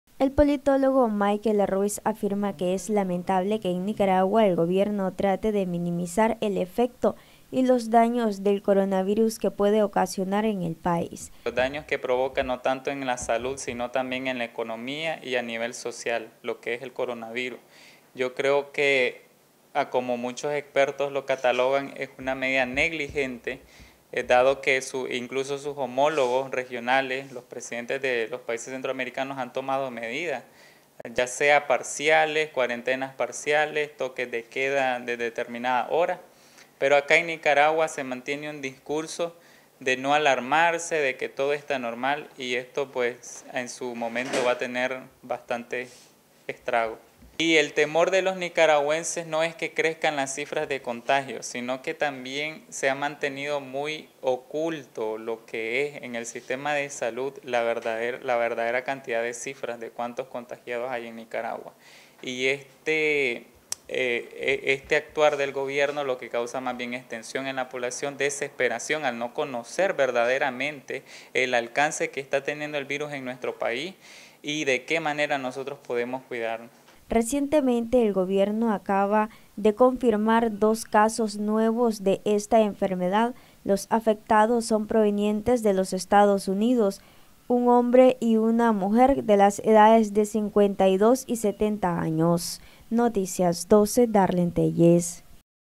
“El gobierno trata de minimizar los daños que pueda causar el COVID-19 en el país”
El politólogo Michael Ruíz, dio a conocer que el gobierno de Nicaragua, trata de minimizar los daños que pueda causar en coronavirus en la nación.